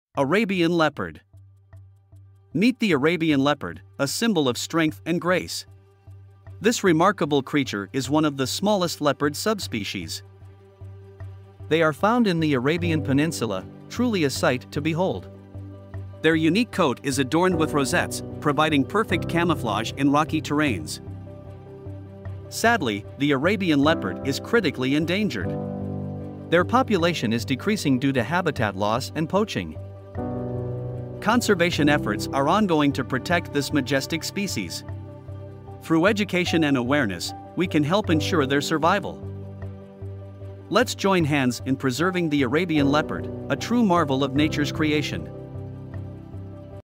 Guardians of the Mountains: The Last Stand of the Arabian Leopard
The Arabian leopard (Panthera pardus nimr) is a critically endangered subspecies of leopard native to the Arabian Peninsula in Southwest Asia. It is a medium-sized big cat characterized by its pale yellow coat with distinctive rosette-shaped markings. Arabian leopards are highly elusive and primarily inhabit mountainous and hilly terrains, as well as rocky caves and ravines. Their diet consists mainly of ibex, goats, and other small mammals. Due to habitat loss, hunting, and fragmentation of their population, Arabian leopards face a severe threat of extinction, with only a few individuals believed to remain in the wild. Conservation efforts are underway to protect and preserve this rare and majestic species.